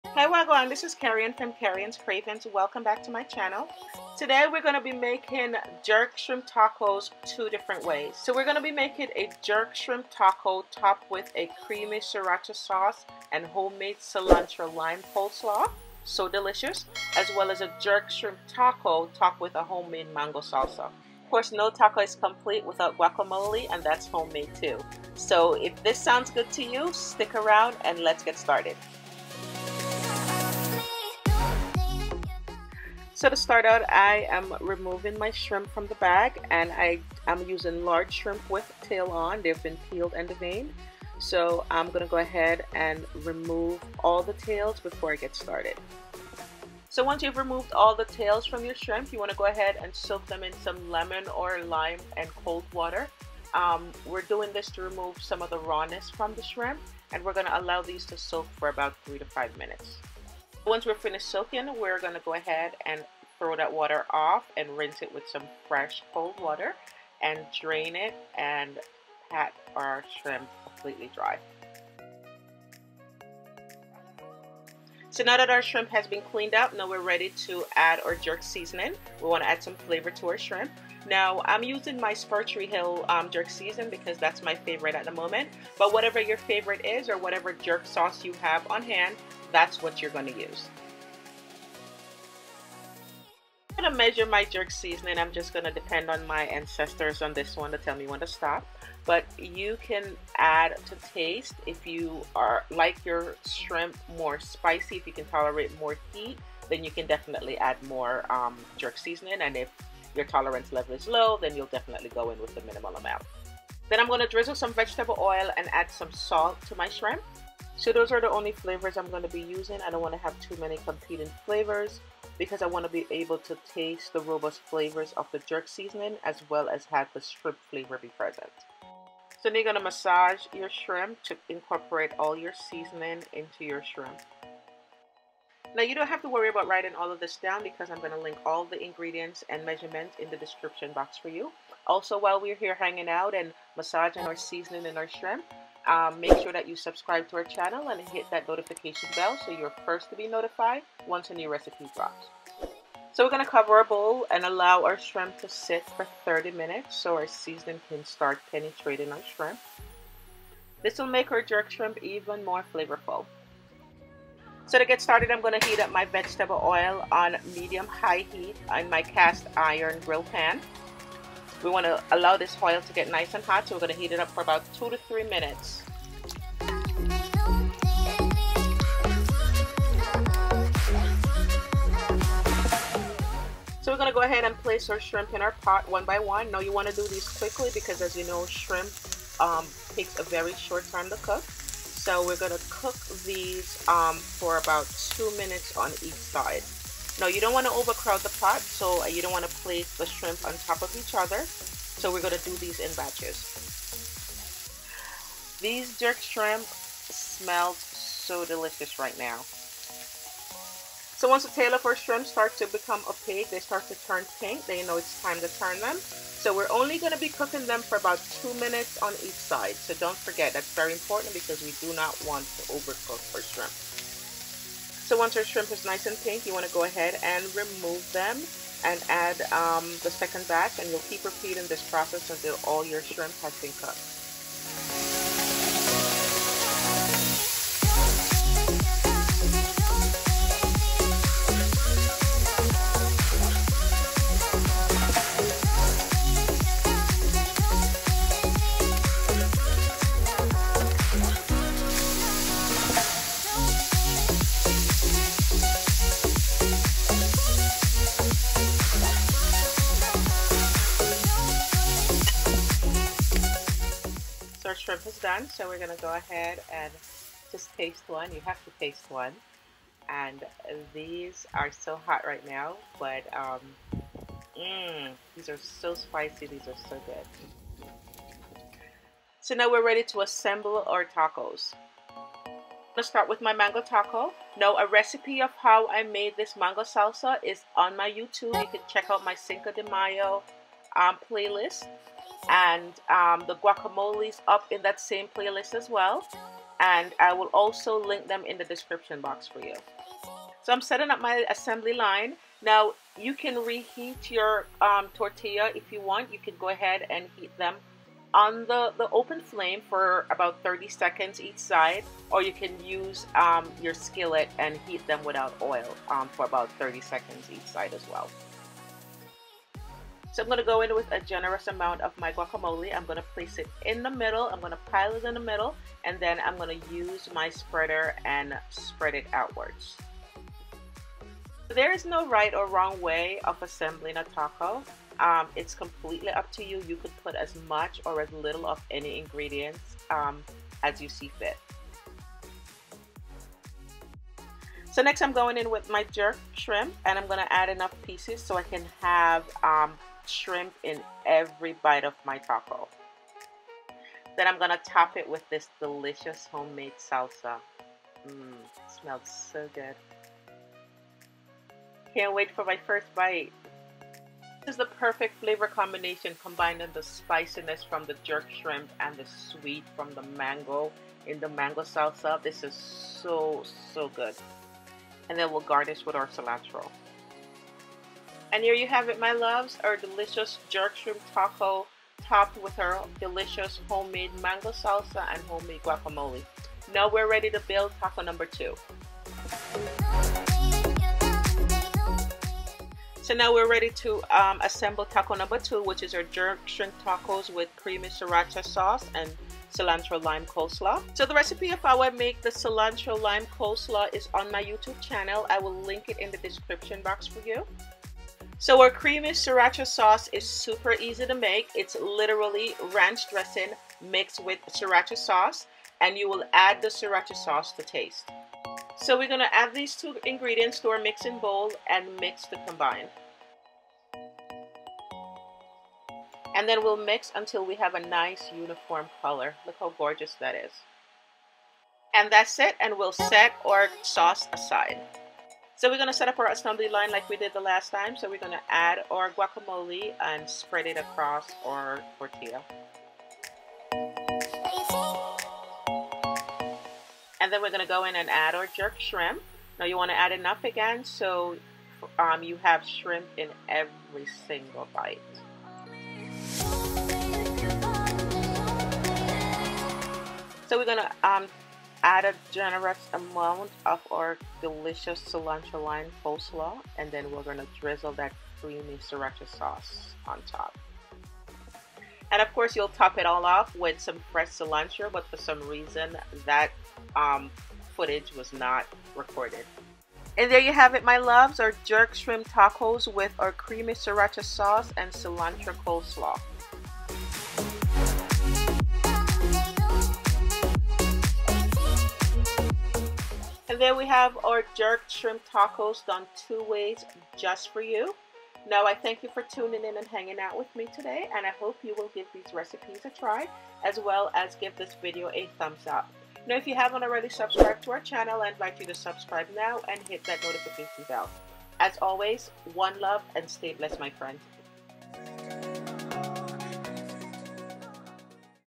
JERK SHRIMP TACOS |TWO DIFFERENT WAYS
This video shows you step by step how to the make the Jerk Shrimp Tacos two different ways at home. Jerk Shrimp Tacos with a homemade mango salsa & home made guacamole & a Jerk Chicken Taco with a Creamy Sriracha Sauce & home made Cilantro Lime Slaw.

RECIPE BELOW 👇🏾👇🏾👇🏾

My username is @kerriannskravings on every platform & @kerriskravings on Twitter!

RECIPE
Here’s what you’ll need to get started:
Jerk Shrimp
1 lb shrimp, peeled and deveined
Jerk seasoning (adjust for heat preference)
1 tablespoon extra virgin olive oil + 1/2 teaspoon for drizzling.
lime juice
6 corn tortillas

Creamy Sriracha sauce
1/4 cup ranch dressing or sour-cream or greek Yogurt
1 tablespoon Sriracha

Recipe Notes:

✶✧ If you make this recipe, make sure you tag us on your Instagram posts & stories 𝐤𝐞𝐫𝐫𝐢𝐚𝐧𝐧𝐦𝐚𝐝𝐞𝐦𝐞𝐝𝐨𝐢𝐭.